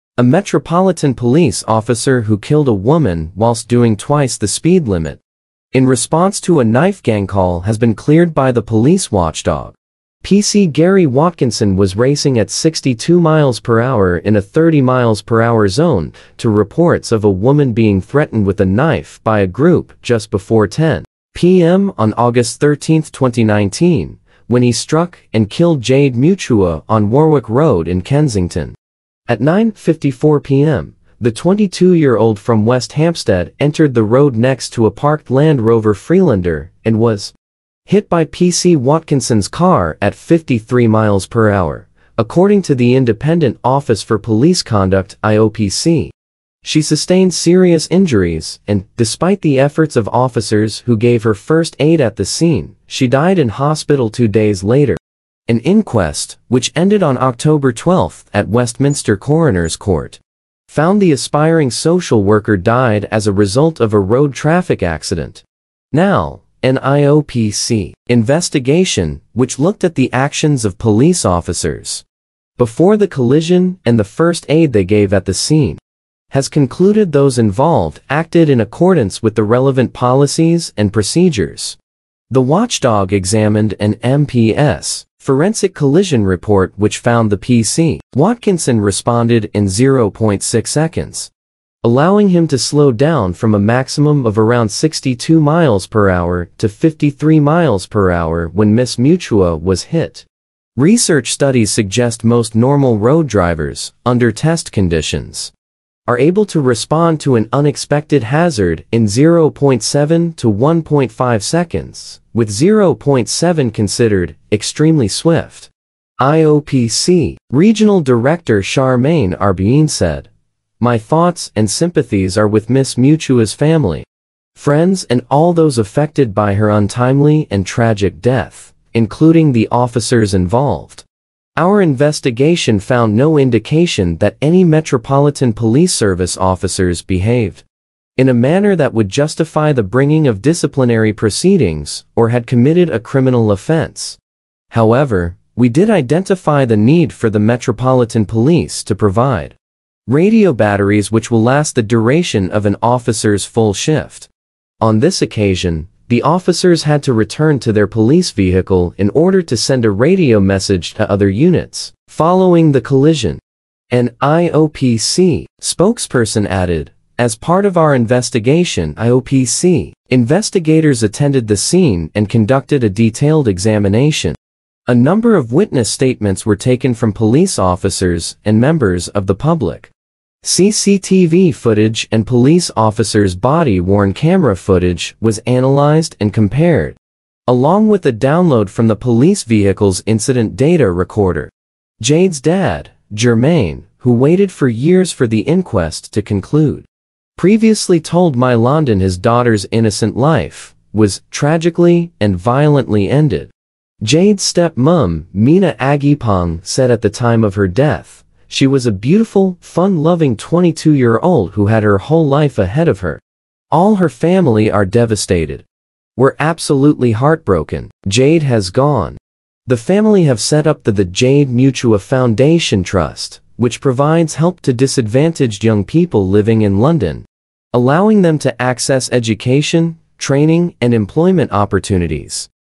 Jade Mutua: Met Police officer 'not at fault' for killing aspiring social worker
Metropolitan Police officer who killed a woman whilst doing twice the speed limit in response to a knife gang call has been cleared by the police watchdog. PC Gary Watkinson was racing at 62mph in a 30mph zone to reports of a woman being threatened with a knife by a group just before 10pm on August 13, 2019, when he struck and killed Jade Mutua on Warwick Road in Kensington.

At 9.54pm, the 22-year-old from West Hampstead entered the road next to a parked Land Rover Freelander and was hit by PC Watkinson's car at 53mph, according to the Independent Office for Police Conduct (IOPC).

She sustained serious injuries and, despite the efforts of officers who gave her first aid at the scene, she died in hospital two days later.

An inquest, which ended on October 12 at Westminster Coroner’s Court, found the aspiring social worker died as a result of a road traffic accident. Now an IOPC investigation, which looked at the actions of police officers before the collision and the first aid they gave at the scene, has concluded those involved 'acted in accordance with the relevant policies and procedures'.